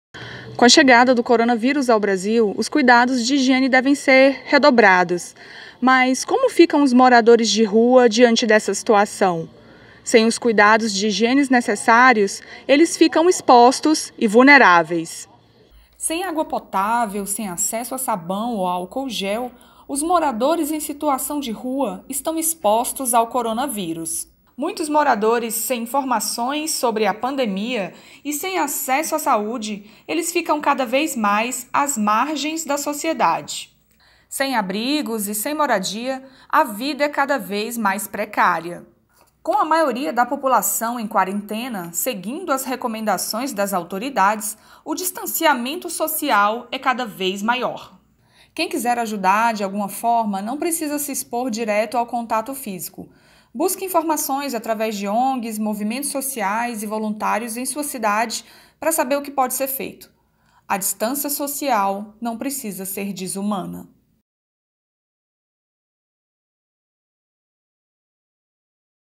A exposição de pessoas em situação de rua a Pandemia do Covid-19
Os que vivem a margem da sociedade estão diretamente expostos ao coronavírus. Pessoas em situação de rua, se tornam esquecidos, abandonados a própria sorte. Nesse momento, a solidariedade pode ser a cura.

Ficha técnica:
Chefe de reportagem: Letícia Fagundes
Repórter: Janara Rodrigues
Edição: Juliana Monaco